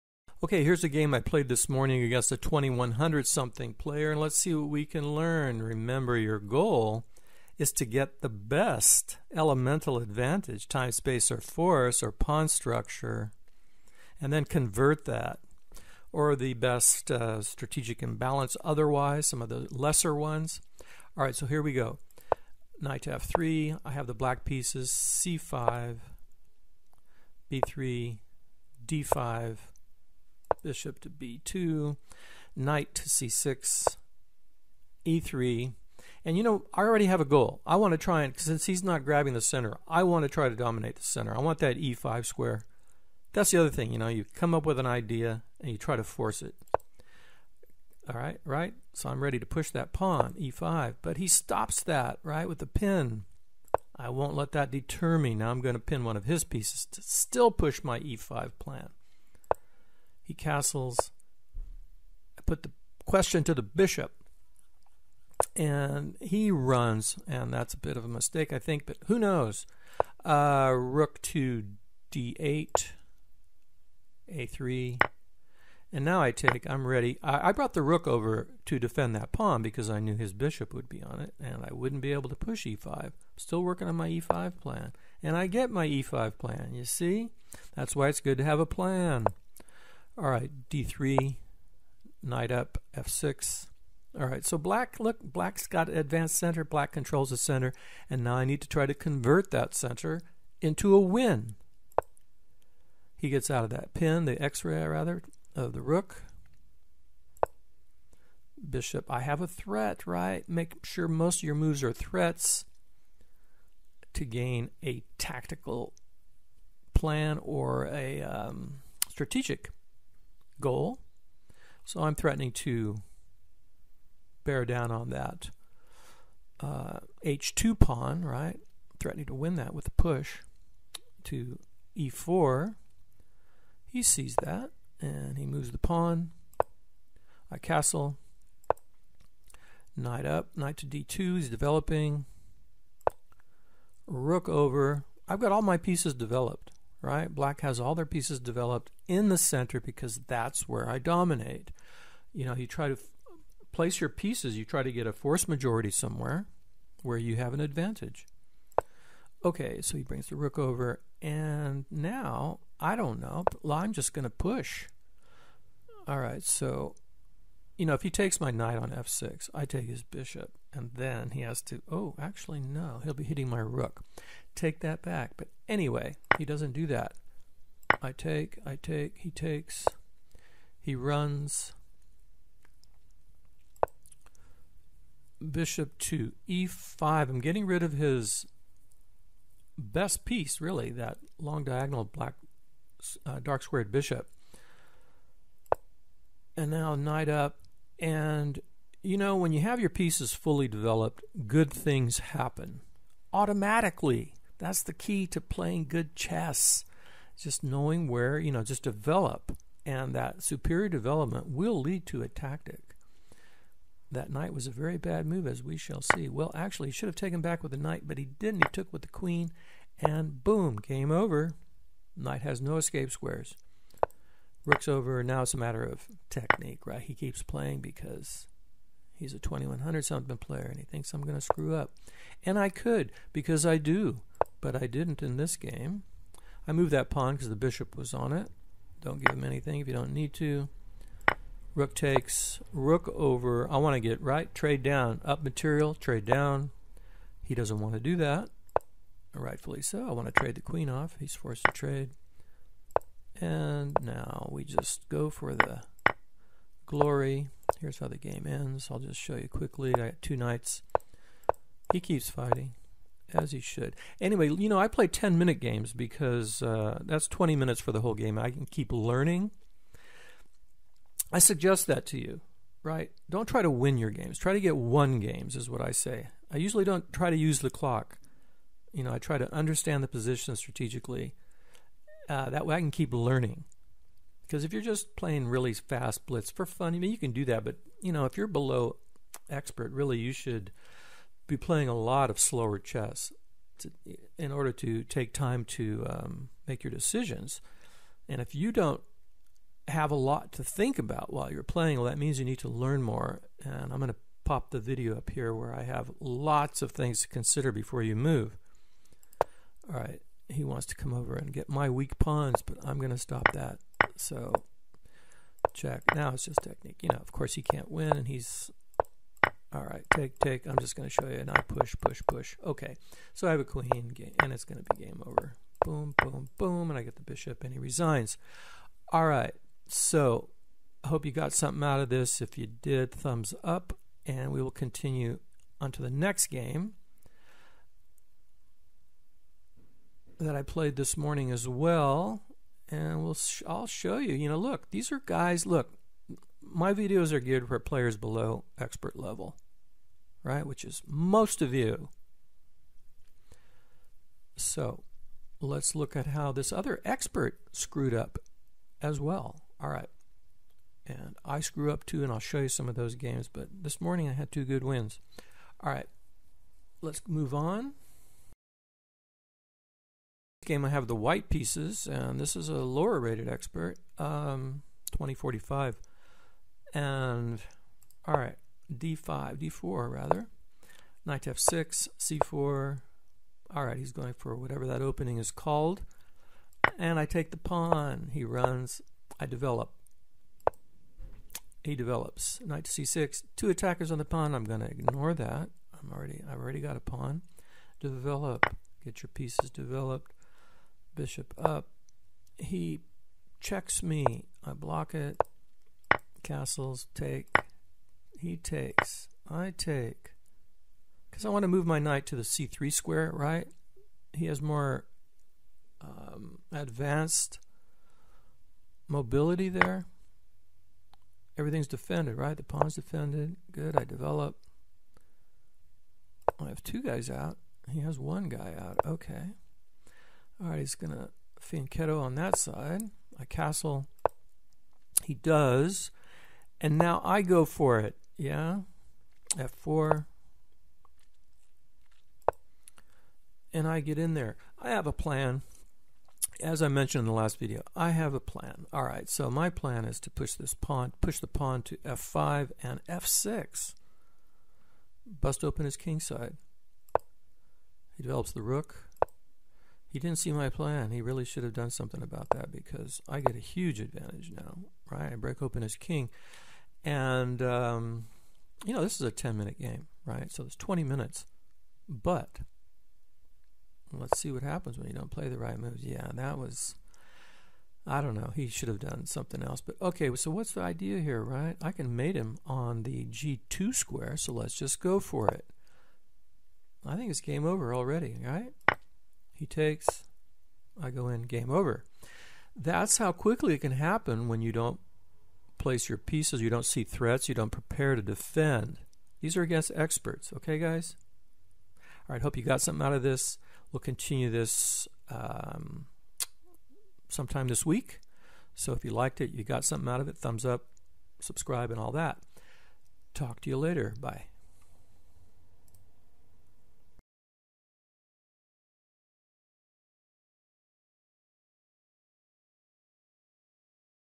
Morning Chess: Why you lose!
Donate $5.00 or more if you like :-) and receive the book, club membership and exclusive content.
I process these by hand so it may take a few minutes to a few hours depending on time zone.

A new series of my morning 10 minute games, where I point errors in thinking and decision making in chess.